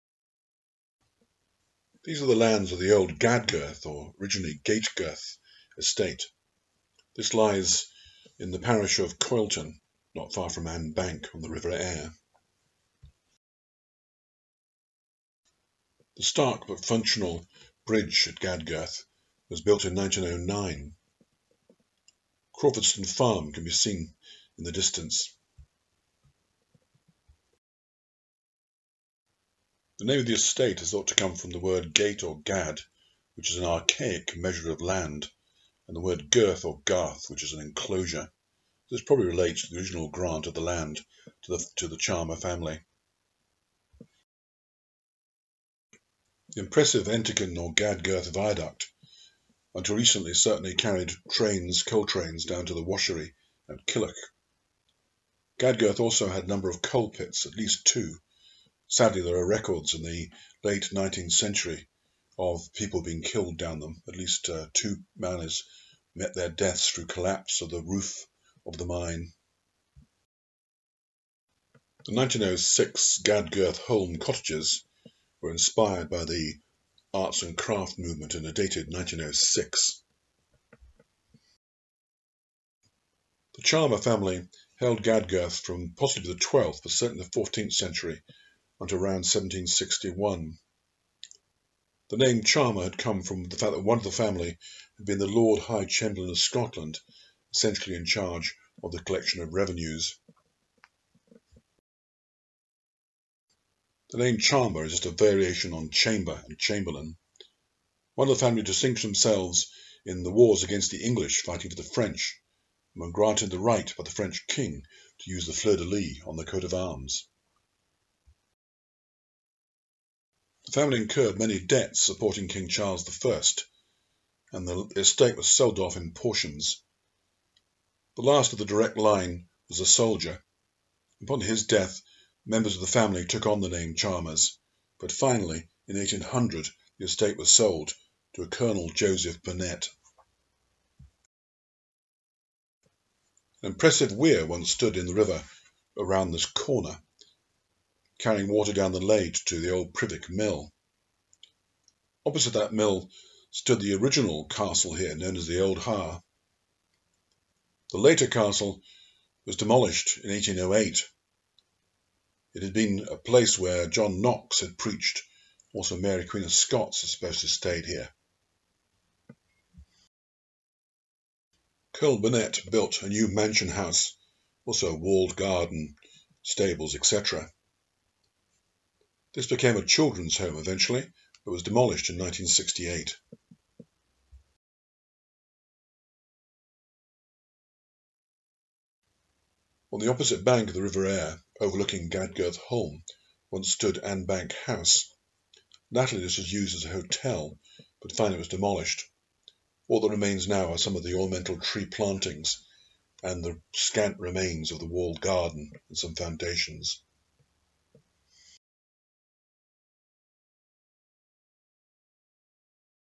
The Gadgirth Estate and the River Ayr, Annbank
The Chalmers family held Gadgirth for many centuries, selling to Colonel Joseph Burnett in 1800 after the direct male line had ended. The original castle, known as the 'Old Ha' stand on a whinstone promontory downstream from the later castle demolished by Burnett in 1808, his mansion being demolished in 1968 after having been purchased by the council and acting as a children's home.

 An obelisk stood on the hill above, close to the two shallow Gadgirth Coal Pits, scene of at least two miners deaths from rock falls. John Knox once visited the castle and Mary Queen of Scots is said to have stayed here. Annbank House overlooked Gadgirth Holm until it was demolished. The 1906 Gadgirth Holm Cottages were inspired by the arts and crafts movement.